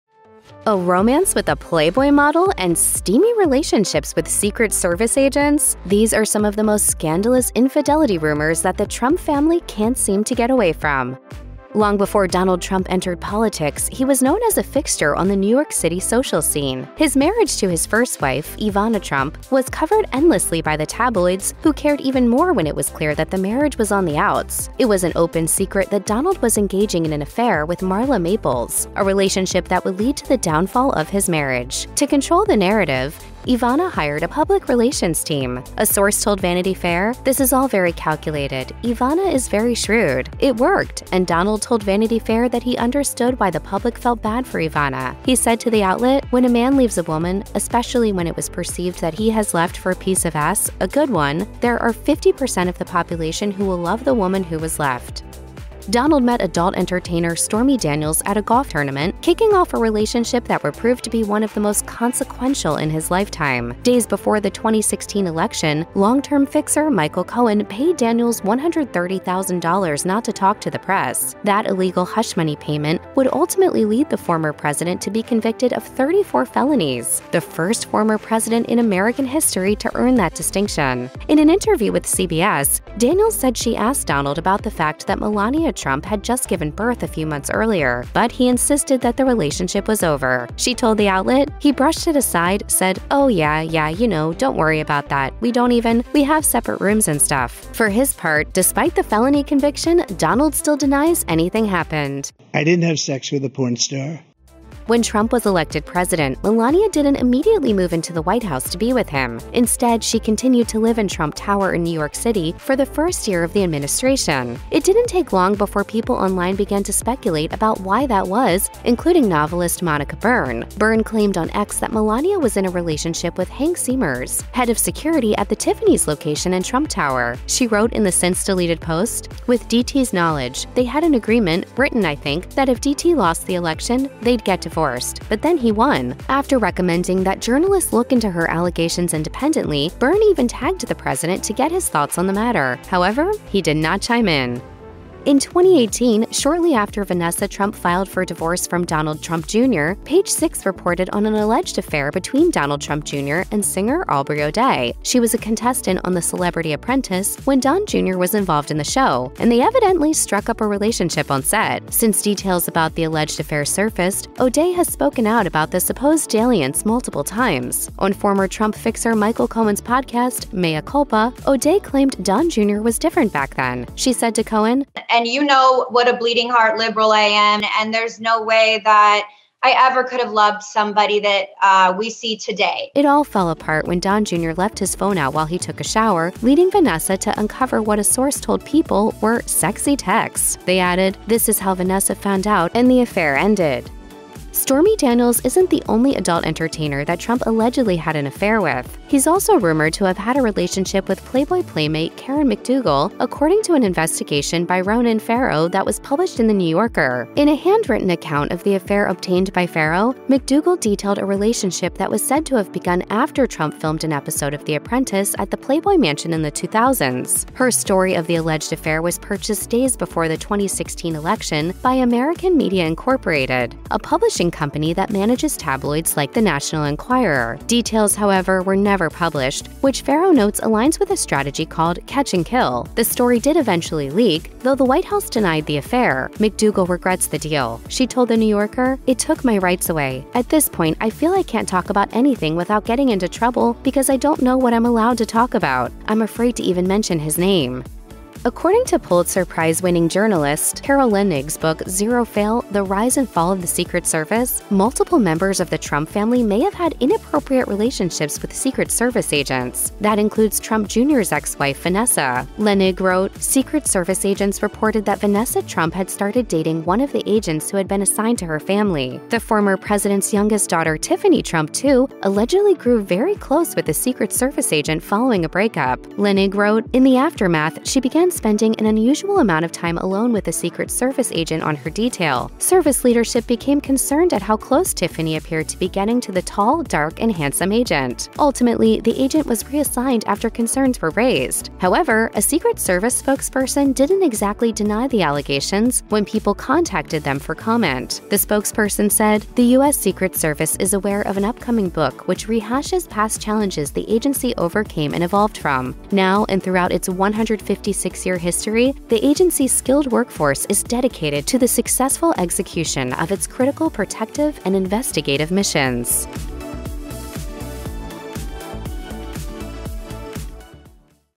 The Affair Rumors Still Haunting The Trump Family
A romance with a Playboy model and steamy relationships with Secret Service agents?? These are some of the most scandalous infidelity rumors that the Trump family can't seem to get away from.

Trump and Marla Maples | 0:00
Trump and Stormy Daniels | 0:55
Melania and a Trump Tower employee | 1:44
Donald Trump Jr. and Aubrey O'Day | 2:25
Trump and Karen McDougal | 3:14
Trump women and Secret Service agents | 4:08

Voiceover By: Chrissy Baker